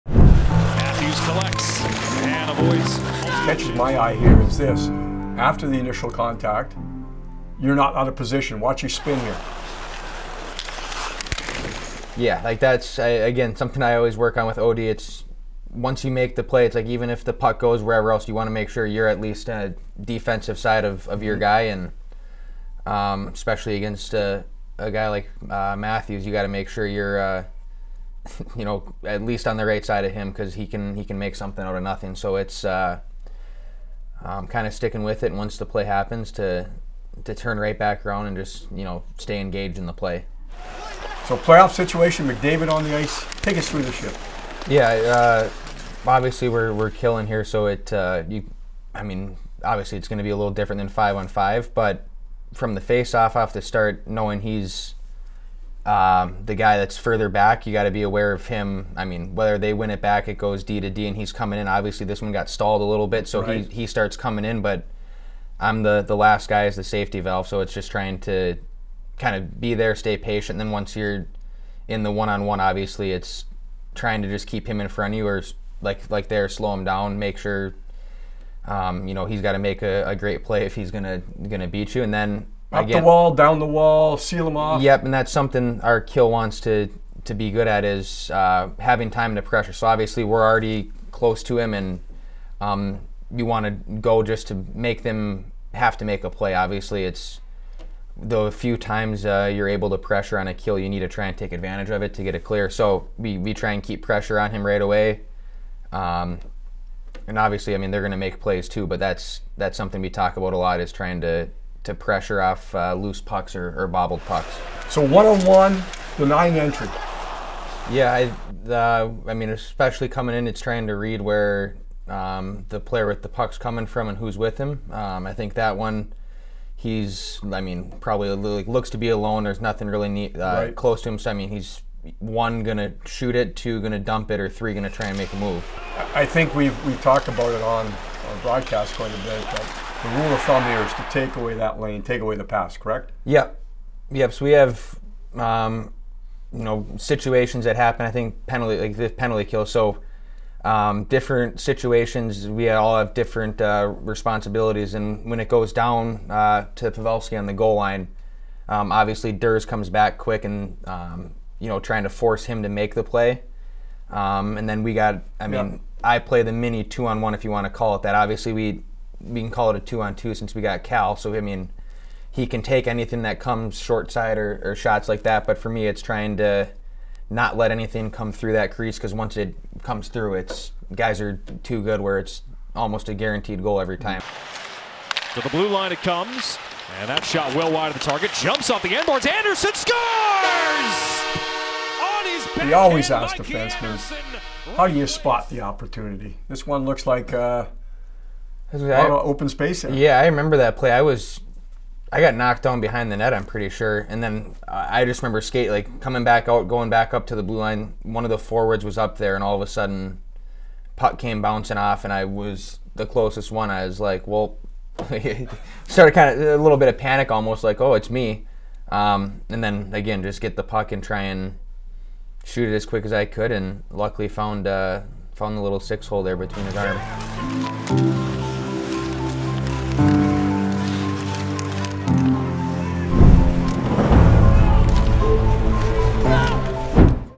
LA Kings Defenseman Mikey Anderson breaks down his best plays | Inside the Play by Pepperdine
LA Kings Defenseman Mikey Anderson sat down with LA Kings Alumni and Current Bally Sports West Analyst, Jim Fox, to break down the best plays of his Kings career.